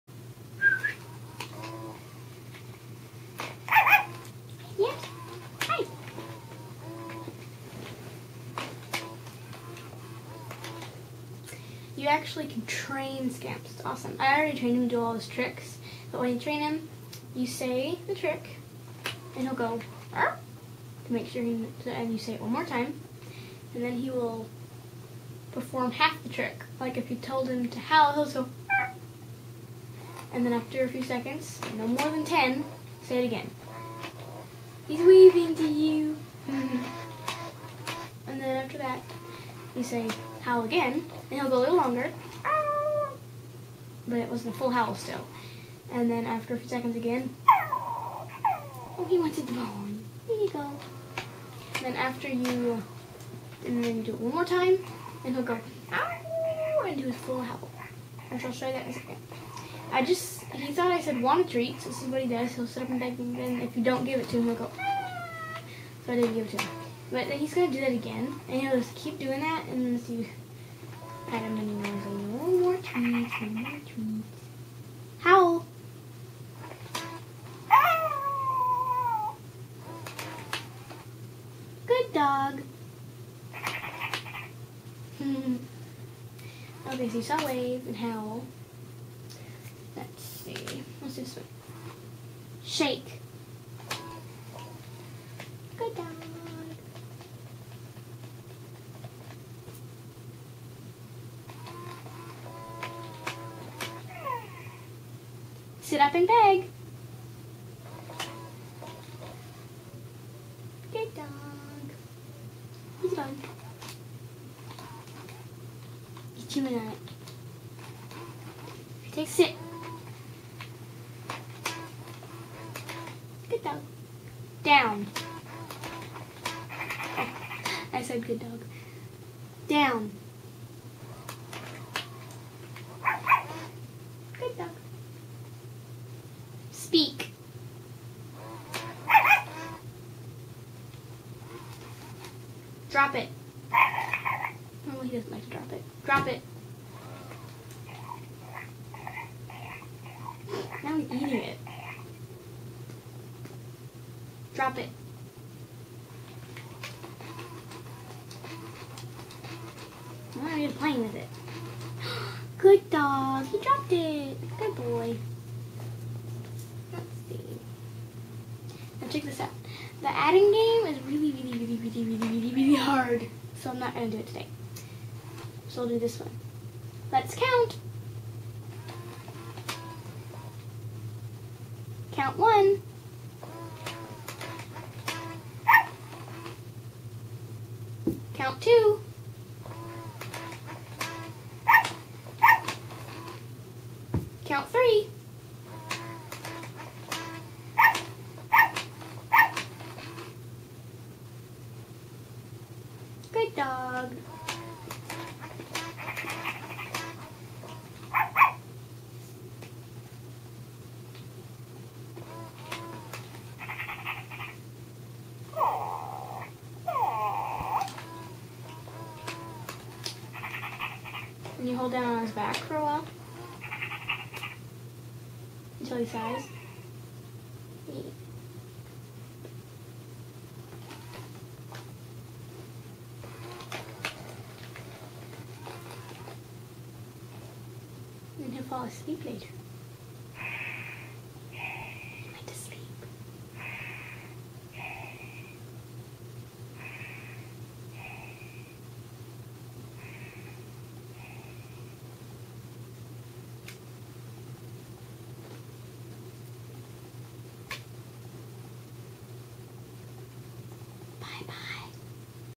Furreal Friends Scamps My Playful Pup Demonstration
A demonstration of the Furreal Friends Scamps My Playful Pup.

Sorry about how interruptive Scamps is! What I am trying to say is that to teach him a command you have to, first, say the command once and wait for him to make a confused noise and tilt his head to one side. Then say the command again and he will perform only part of the trick. Say it again and he will perform a little more. Say it one more time and he will perform the whole trick and if you ever say it again, he will still do the whole thing. If you want to re-teach his commands, just turn him off and then back on again.
He comes with:   a bone, toy, and instruction manual.
Scamps responds to:   touch and sound
He obeys the commands:   sit, down, speak, good dog, wanna treat, hey Scamps, hey puppy, and drop it.
You can teach him:   gimmy a paw, shake, wave, howl, sit up and beg. You can teach him to count and add, but those are very hard to teach.
He responds to your touch in different places:   his back, under his left ear, and under his right ear.